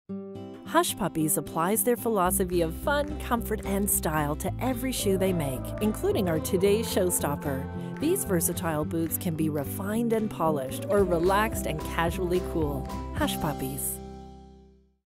Hush Puppies October 2013
Promo for Hush Puppies at the Shopping Channel, October 2013.